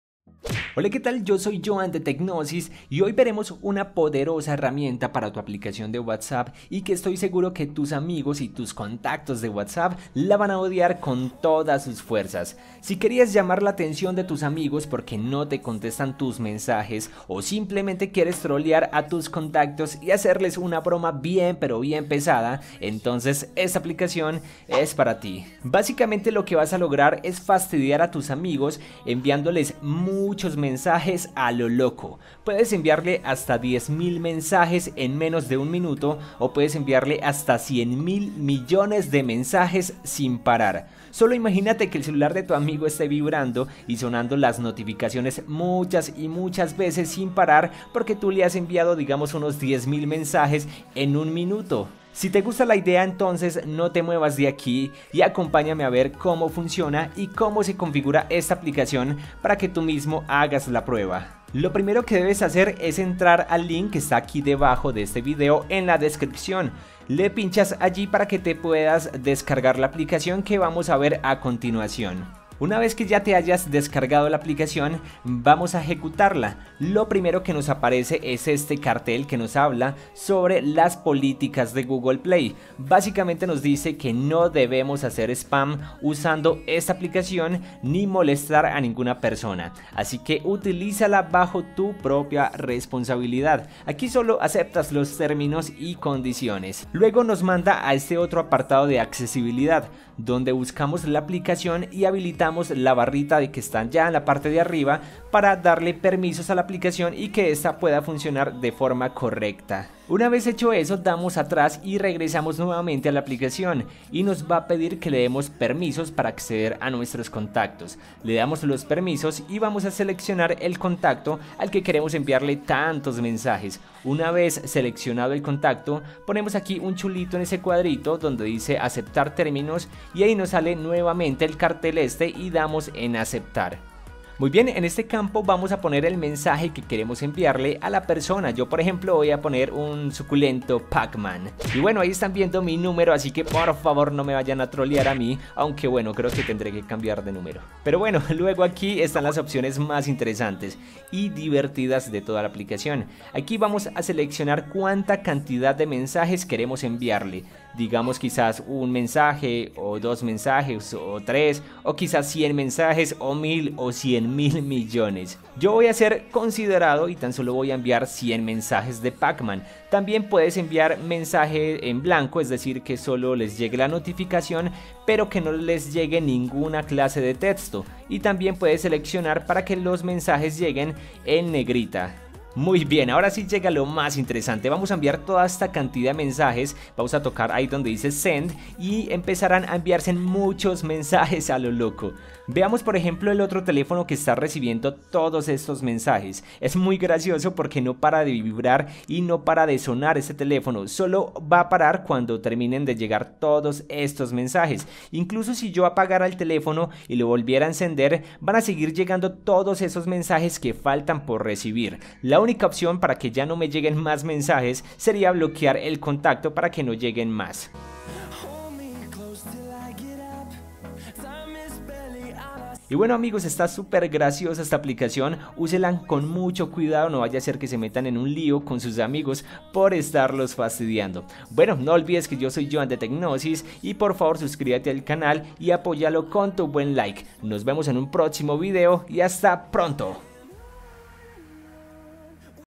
La FUNCIÓN OCULTA QUE WHATSAPP TE PROHÍBE USAR
Aquí les comparto esta función para la aplicación de whatsapp úsala con mucha responsabilidad y descargarla gratis solo para que la conozcas :v

pdta: ya elimine mi numero de whatsapp para que no me molesten a mi :v